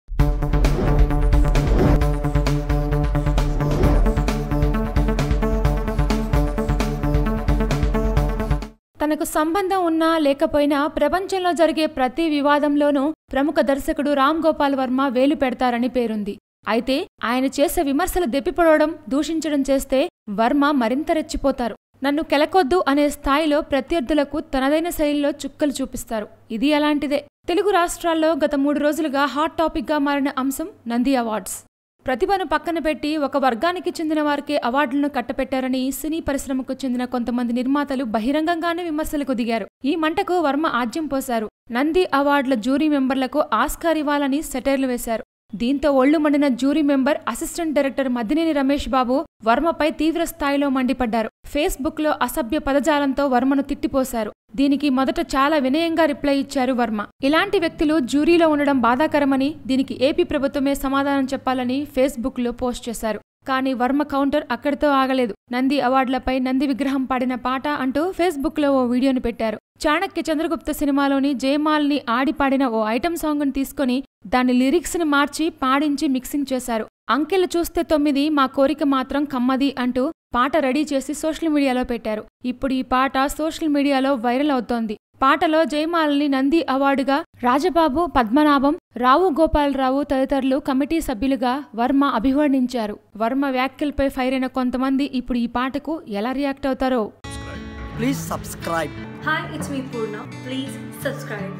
Ram Gopal Varma Parody Song On AP Nandi Awards Went Viral | Latest Telugu Cinema News
Watch: Ram Gopal Varma  Parody Song On AP Nandi Awards Went Viral